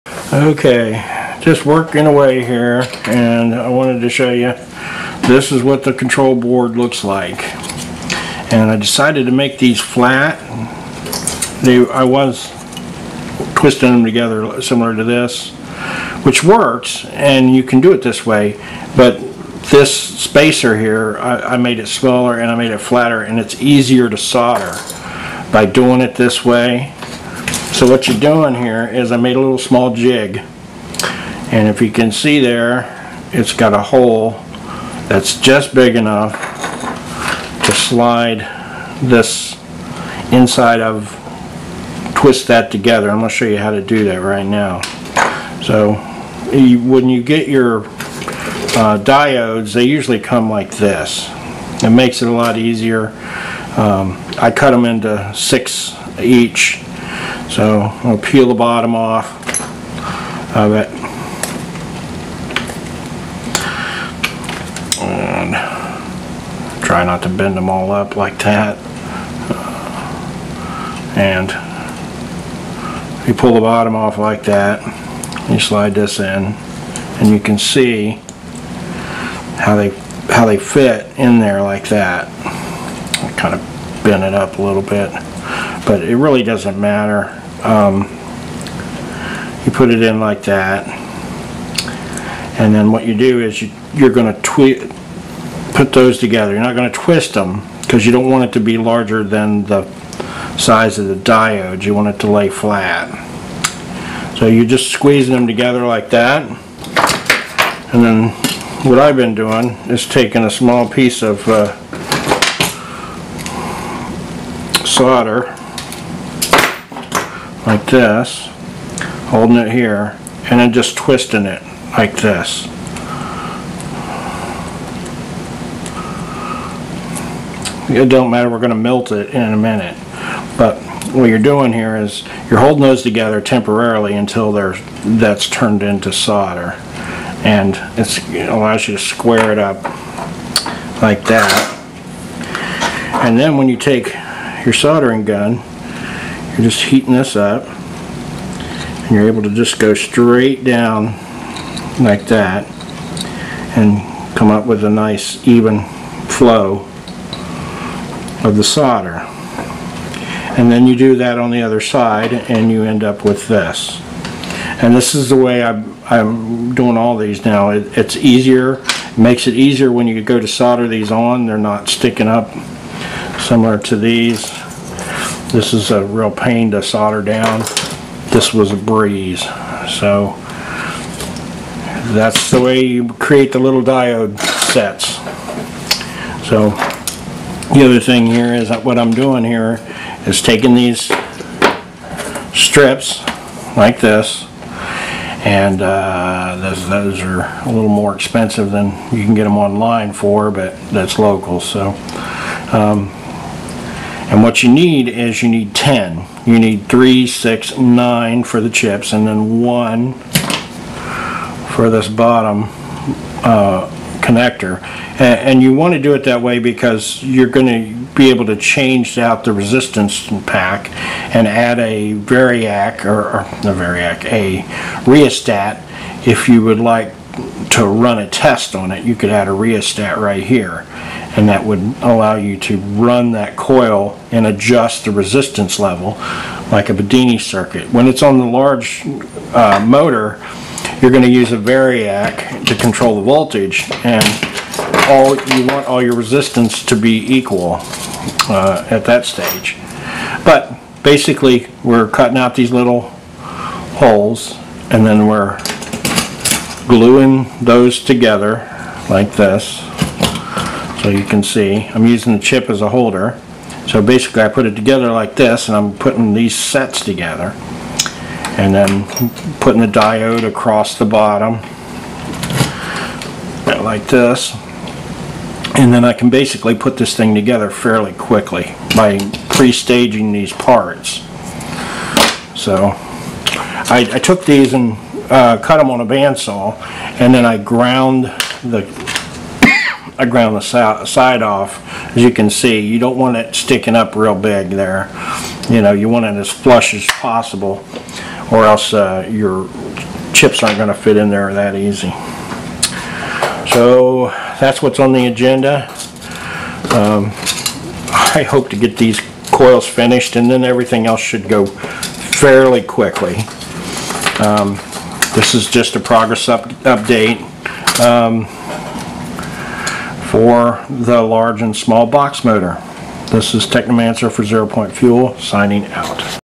Coil Build for Boxmotor
Update Boxmotor building coils and prepping parts